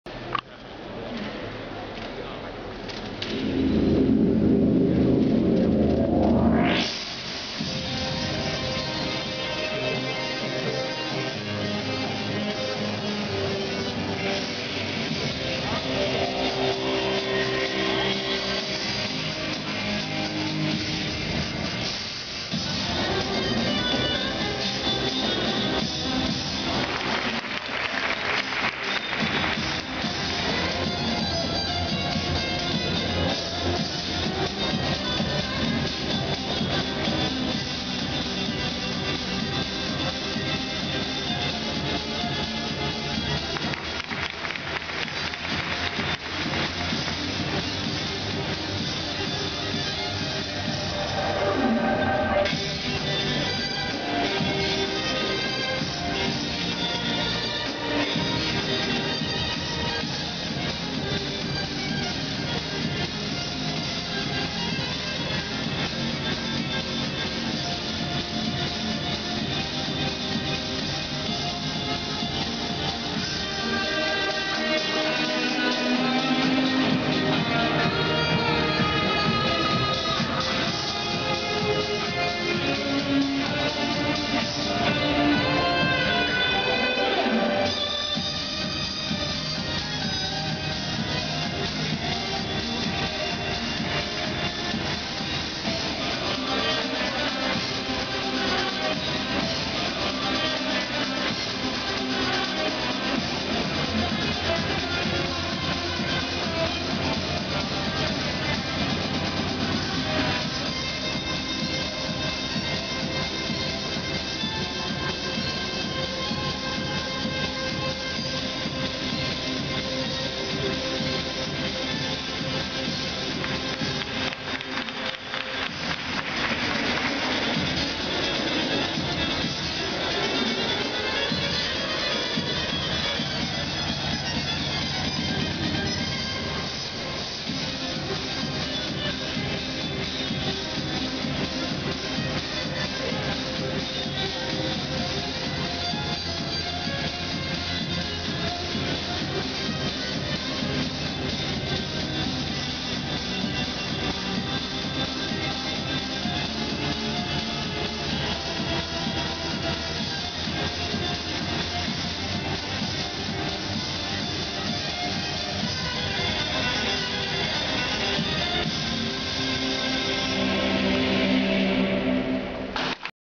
2009 Nebelhorn Trophy - Lejeanne Marais SP
Lejeanne Marais of South Africa
Short Program at 2009 Nebelhorn Trophy in Oberstdorf
TSS 34.86 = TES 20.66   PCS 14.20
home video